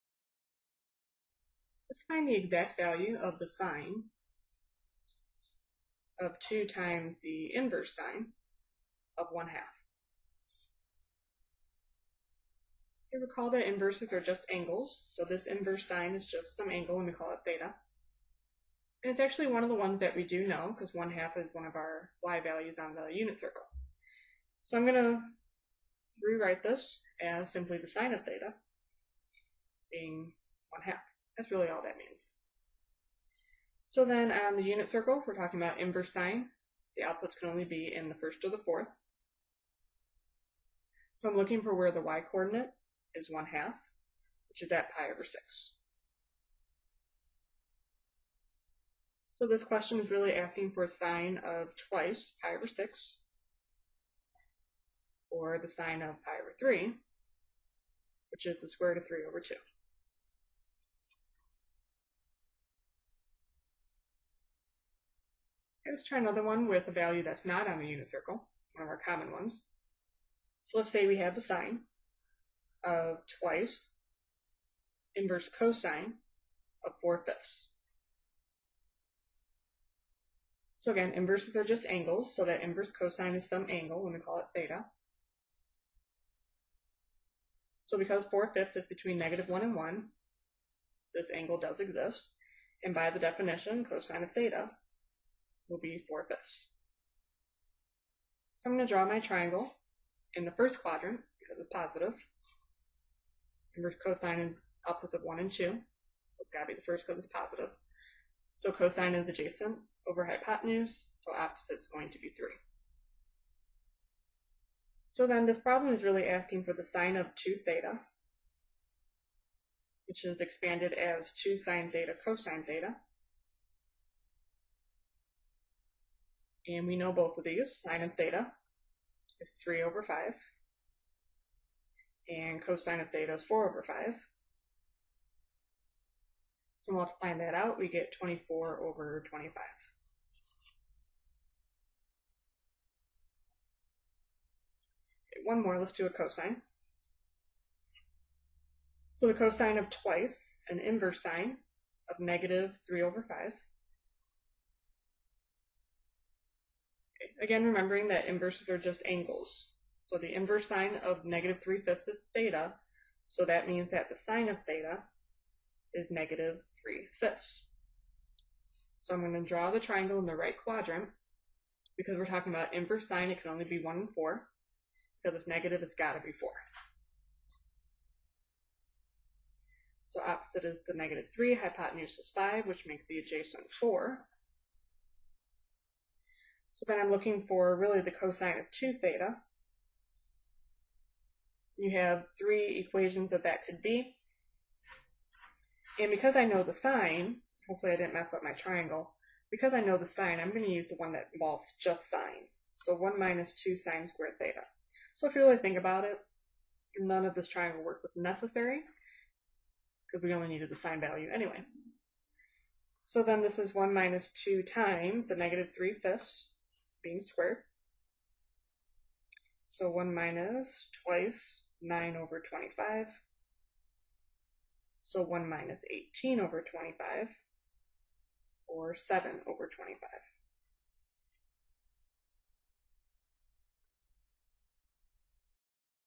Double angle formulas and inverses
(a) sin[2arcsin(1/2)]
(b) sin[2arccos(4/5)]